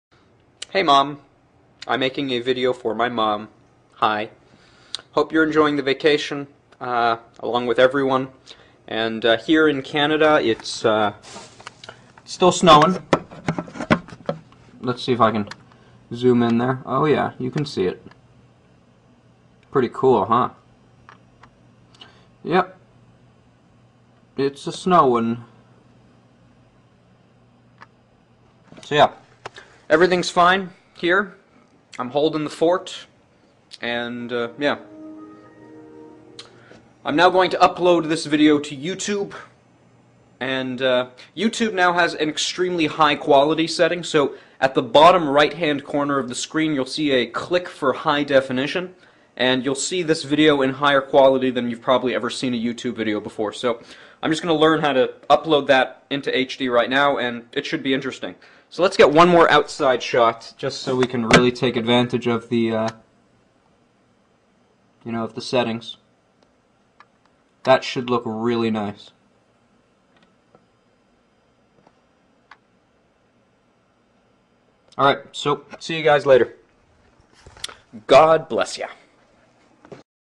mom
Venomfangx makes a video for his mom.

And leaks out some interesting info.